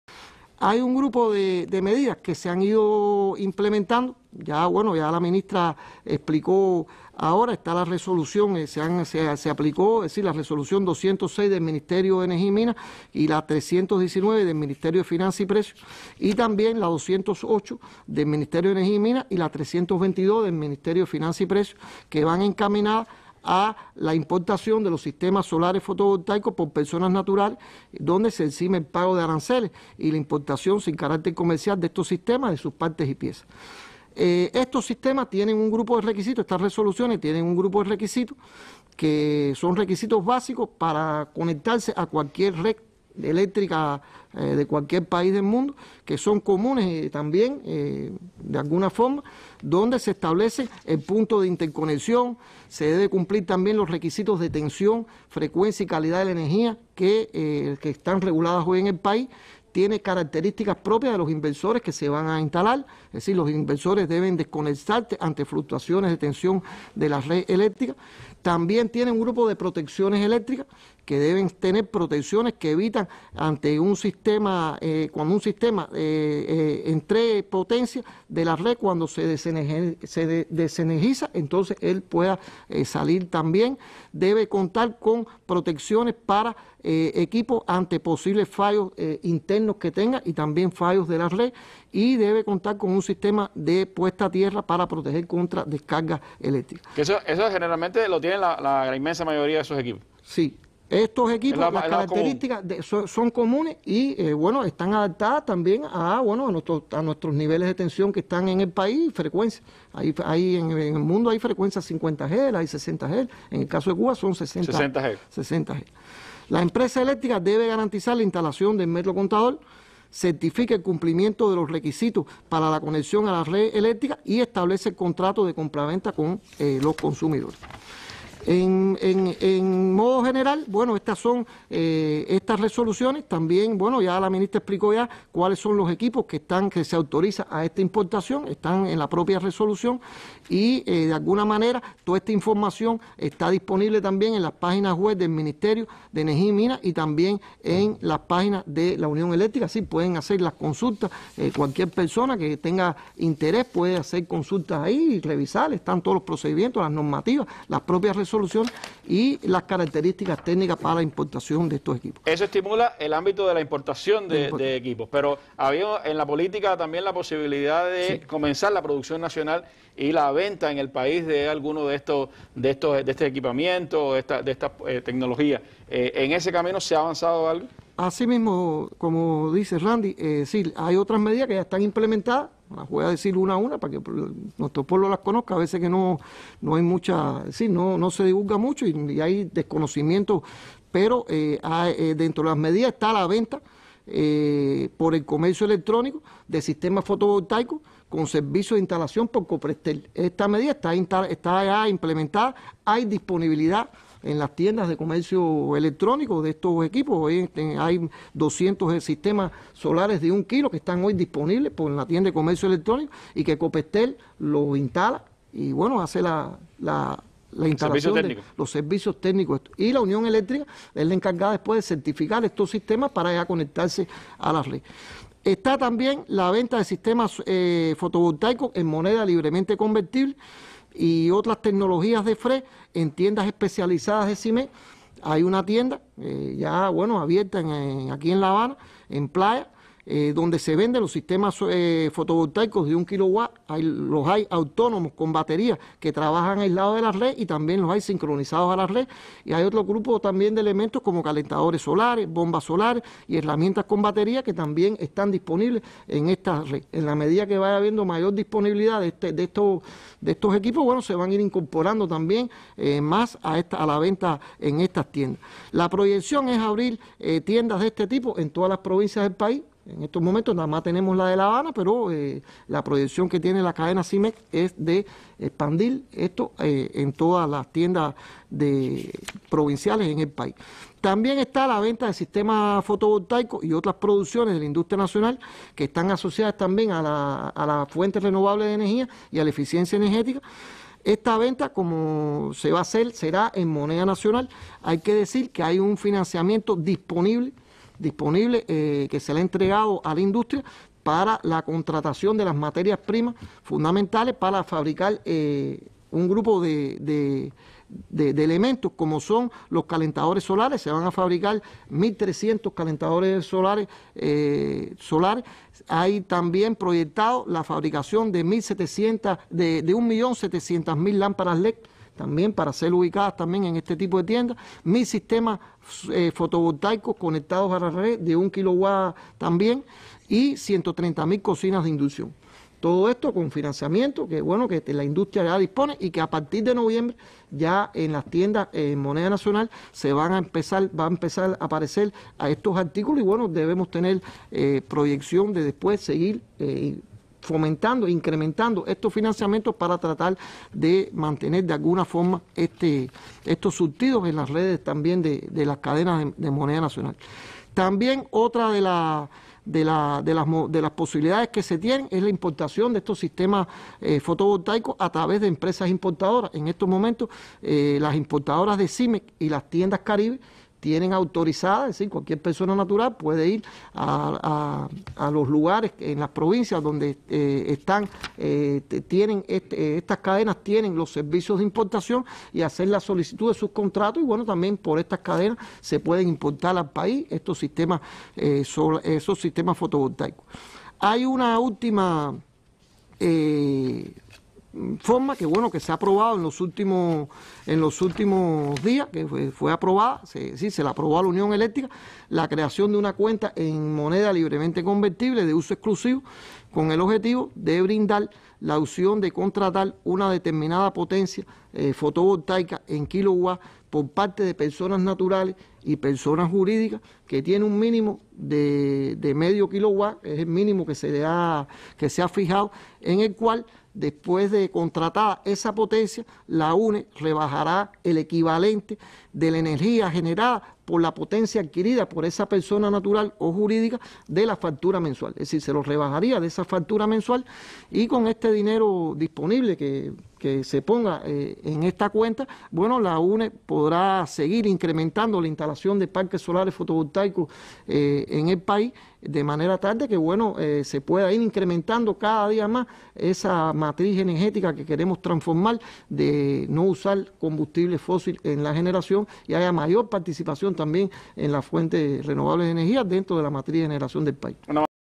¿Cuál es la estrategia de Cuba para ampliar la comercialización de equipos con energías limpias?
Entre las medidas que se han ido implementando están las resoluciones 206 y 208 del Ministerio de Energía y Minas y la 319 y la 322 del Ministerio de Finanzas y Precios, encaminadas a la importación de los sistemas solares fotovoltaicos por personas naturales, con exención del pago de aranceles, y la importación sin carácter comercial de estos sistemas, de sus partes y piezas. Si quiere conocer otras vea el video completo, explica el ministro de Energía y Minas, Liván Arronte Cruz.